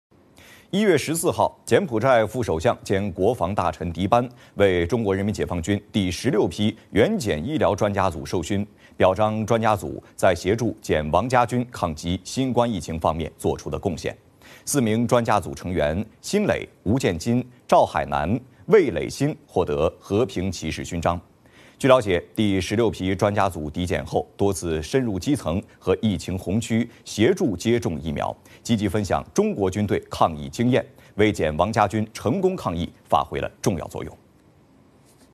中国援柬军医专家组获柬埔寨“和平骑士勋章”
1月14日，柬埔寨副首相兼国防大臣迪班为中国人民解放军第16批援柬医疗专家组授勋，用来表彰专家组在协助柬王家军抗击新冠疫情方面作出的贡献。